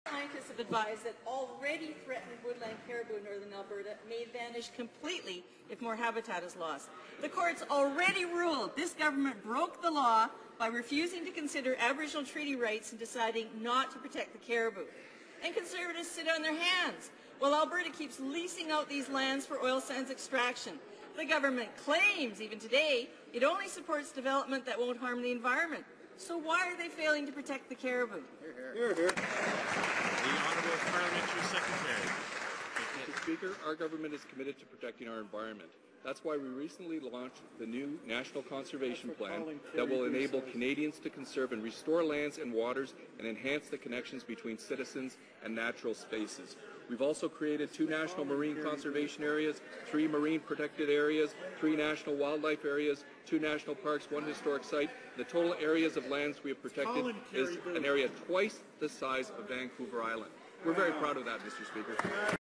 Question Period - Northern Alberta caribou
The government claims, even today, that it only supports development that will not harm the environment. Therefore, why is it failing to protect the caribou?